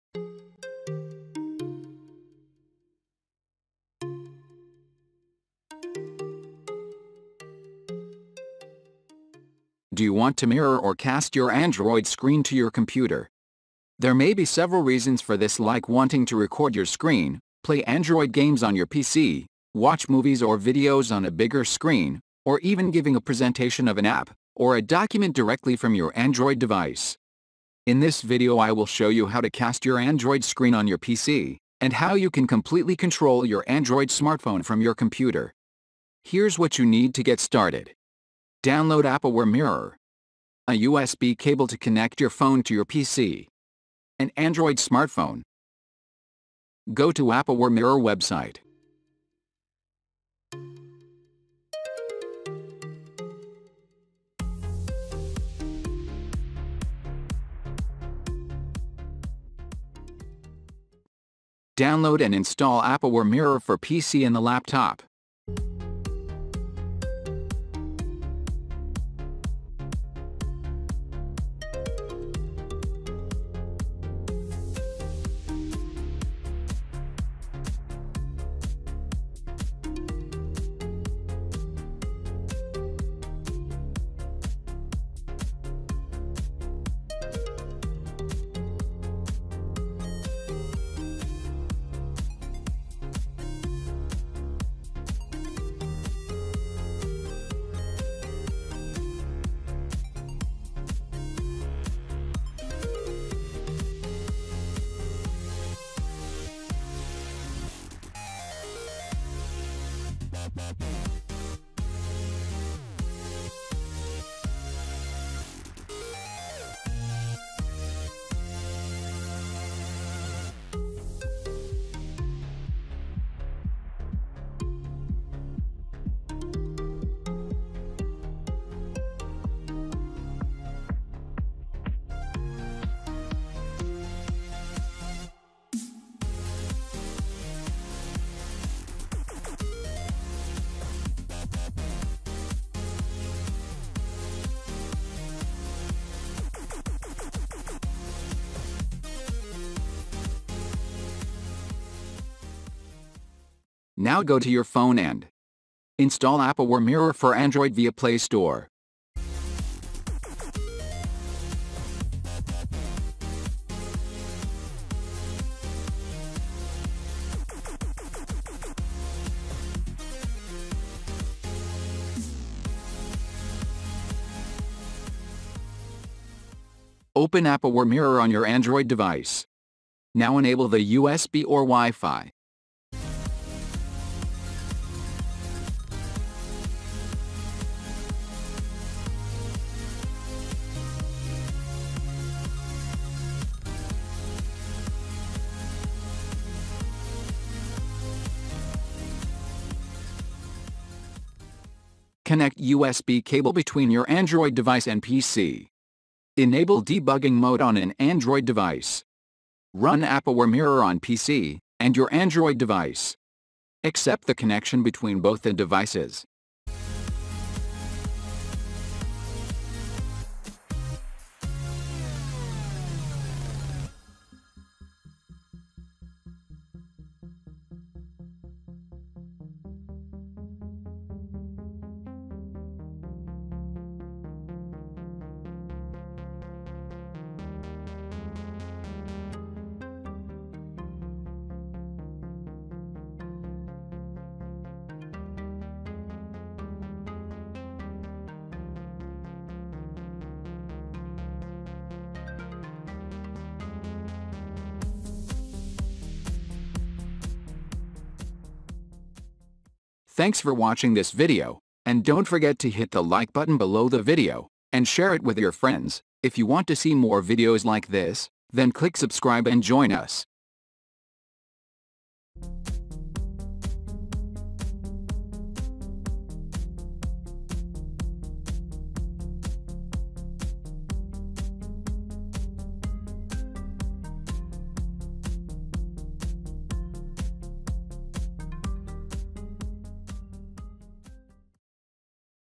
How to Mirror your Android Screen to PC Laptop
Do you want to see how to Display your Mobile Phone Screen on Windows PC laptop using USB or Wireless easily. In this video I will show you how to cast or mirror Android screen on your PC or smart TV, and how you can completely control your Android smartphone from your computer screen mirroring supported mobile phones.

There may be several reasons to stream android screen mirroring if wanting to record your screen, play android games on your PC, watch movies or videos on a bigger screen, Samsung screen mirroring, or Transfer files from your Android phone to your PC. Next video will be how to Screen Mirror Android Phone to TV for Free.